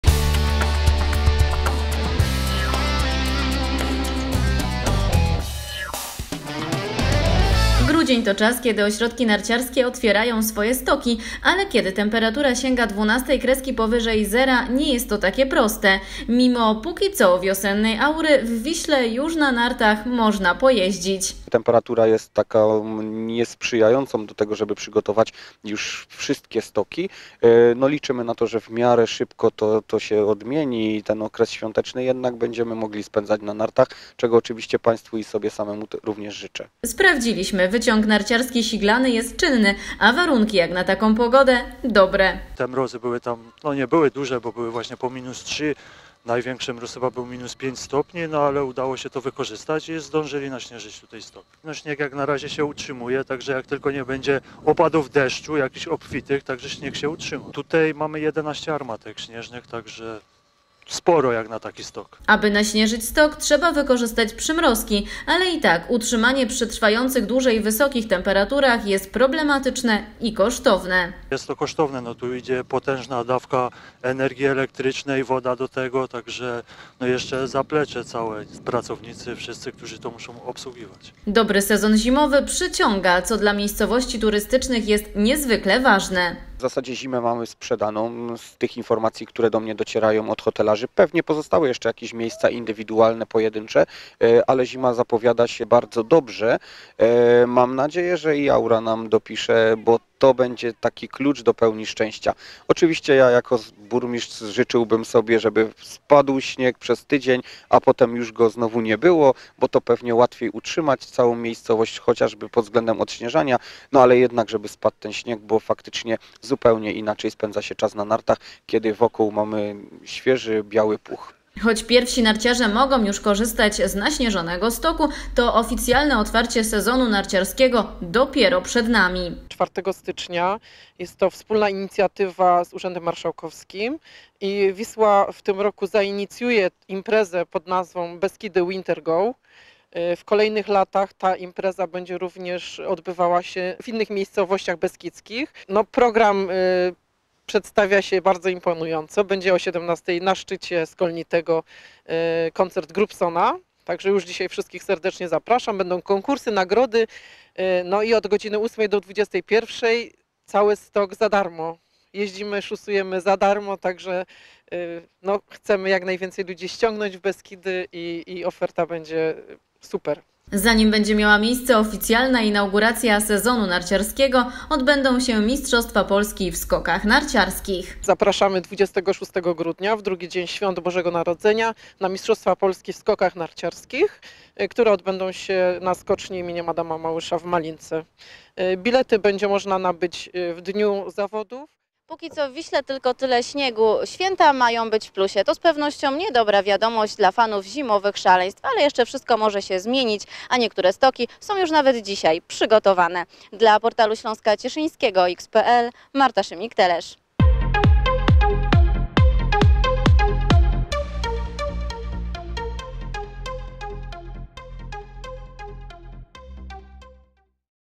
Trudne początki sezonu narciarskiego...
Do świąt Bożego Narodzenia pozostało kilka dni, a pogoda jest raczej wiosenna niż zimowa. 12 kresek powyżej zera jednych cieszą, innych martwią. Wysoka temperatura nie sprzyja sportom zimowym. Postanowiliśmy sprawdzić, jak sytuacja wygląda w Wiśle.